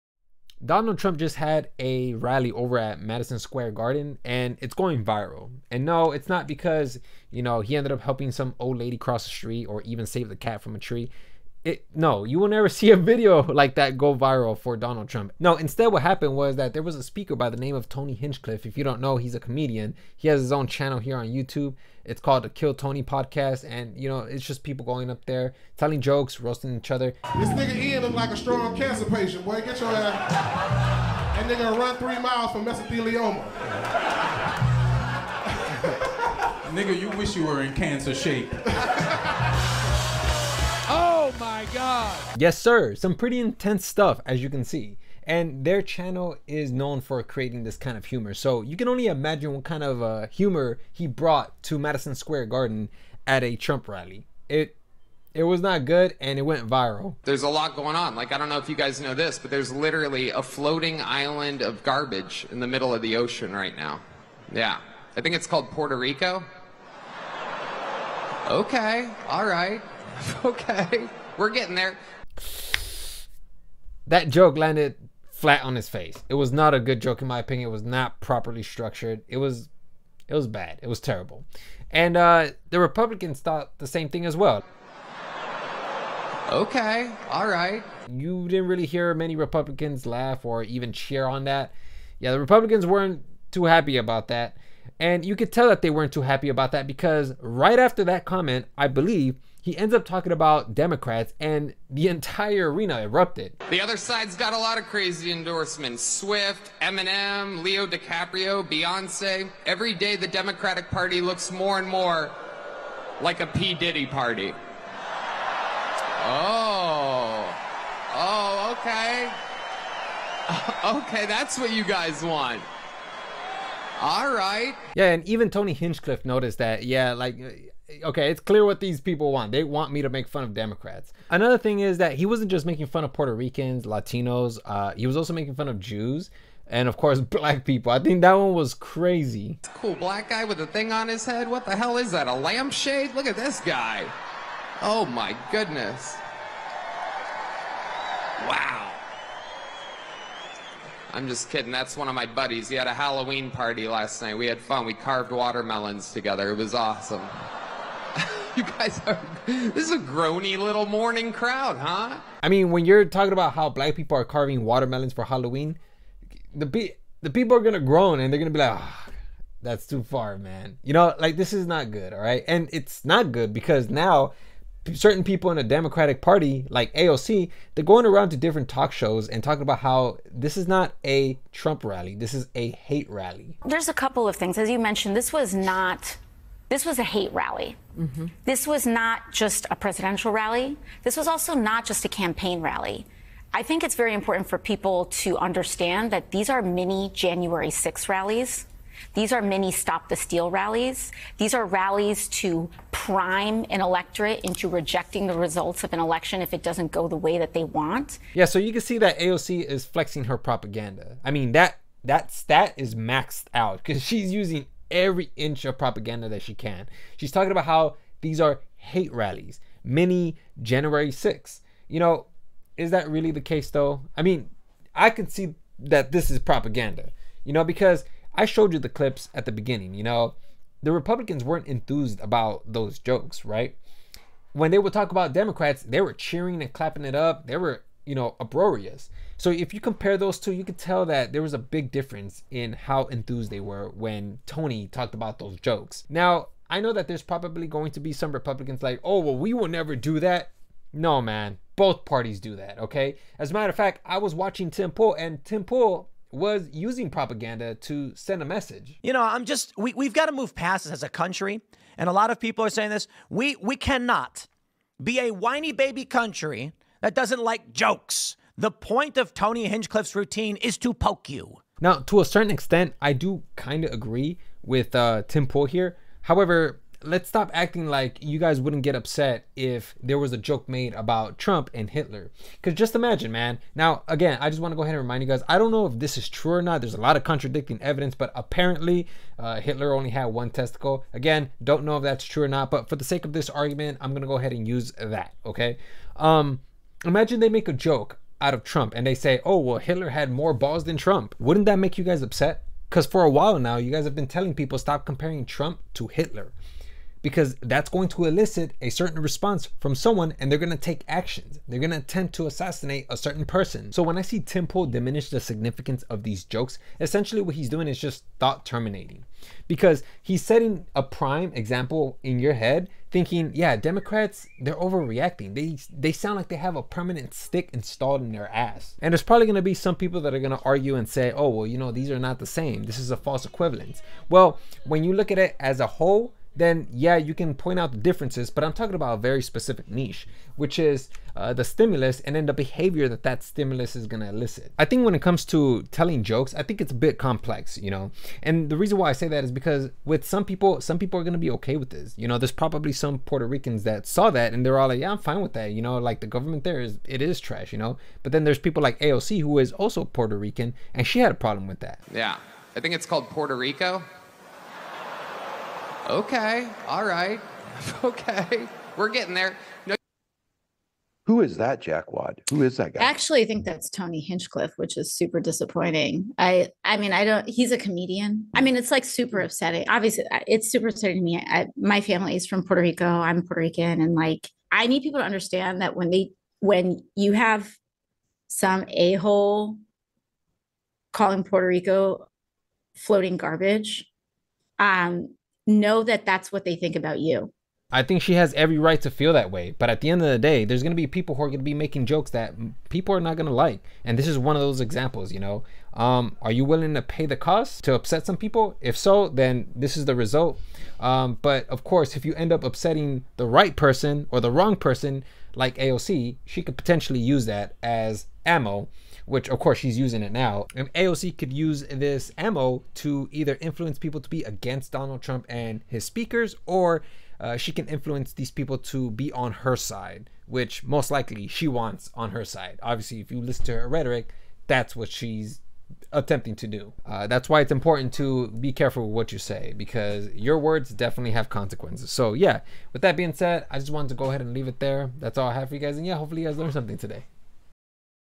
Is This Really Racist?!
Tony Hinchcliffe takes the stage and says a couple of controversial jokes. Lets see what's up